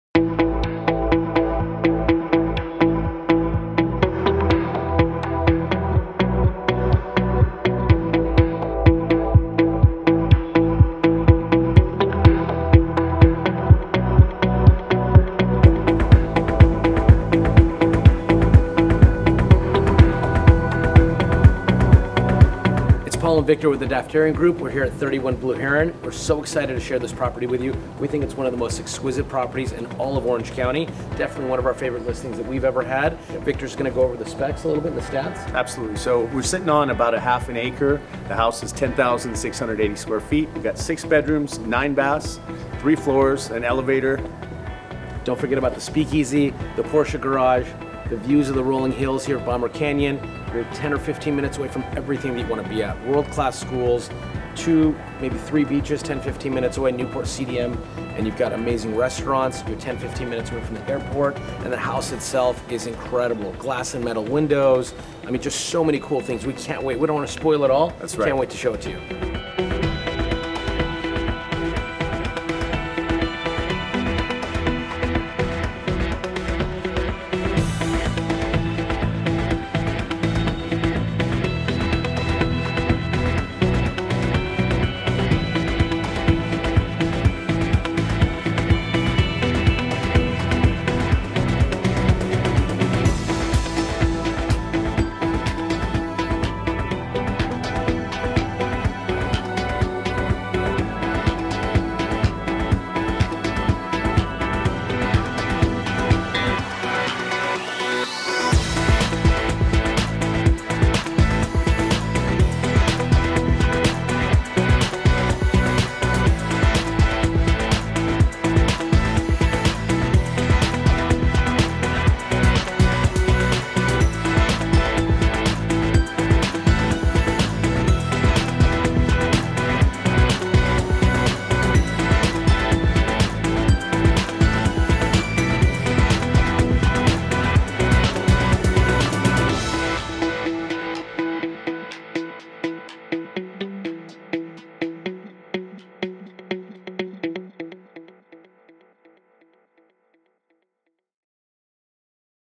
31 Blue Heron | Shady Canyon | Irvine | Daftarian Group Luxury Real Estate
Ideally situated on a premium view lot in the exclusive guard gated community of Shady Canyon, 31 Blue Heron offers views of the rolling hills and dramatic sunsets while offering exceptional design elements that will truly impress design aficionados and the most discerning buyers alike. This stunning custom estate is adorned with steel and glass doors & windows, hand-crafted reclaimed beams, artisan brick walls, Obeche wood cabinetry and paneling, and a mixture of exquisite limestone and wide-plank oak floors just to name a few design elements. The master crafted estate boasts 6 bedrooms + 9 baths offering an elegantly appointed Main Floor Owners-Suite accompanied by a retreat + passthrough fireplace, spa like master bath showcasing artisan level Onyx slab stonework and bespoke millwork along with a "honeymooners grotto". The main floor also offers a designer kitchen with pro series appliances, wood and metal custom Obeche cabinetry, prep kitchen, and direct access to the atrium style breakfast nook and Great room creating a sense of openness and airiness where residents and guests can flow seamlessly between the indoor and outdoor spaces. In addition the main level offers a beautifully appointed formal living room, formal dining space w/ wine cellar and a main-floor secondary guest suite along with an office blooming with natural light and direct access to your private courtyard. Three additional bedroom suites & a loft are located on the upper level of this grand residence. The homes exceptional design continues down into the basement level featuring a oversized home gym w/ glass and mirrored walls, private "speak-easy" bar + second wine cellar along with the ultimate game & media room, a salon, guest suite, powder room, elevator which services all 3 floors, and access to your showcase garage with parking for 7-8 cars. The outdoor spaces in the residence have been professionally re-designed to create a resort like setting all while focusing in on creating the utmost privacy and tranquility. The detailed mosaic tile pool & spa are complimented by two outdoor loggias offering heated floors, an outdoor kitchen, fireplace, TV sitting area, bath, and outdoor shower. The triple gated property offers direct proximity to award winning Irvine schools, Shady Canyon Country Club featuring two world class Tom Fazio golf courses, and minutes away from Orange County's best dining, shopping, beaches and access to multiple nearby freeways.

Listed by Paul Daftarian •DRE #01317949 • Luxe Real Estate
Listed by Victor Jimenez •DRE #01715970 • Luxe Real Estate
CRMLS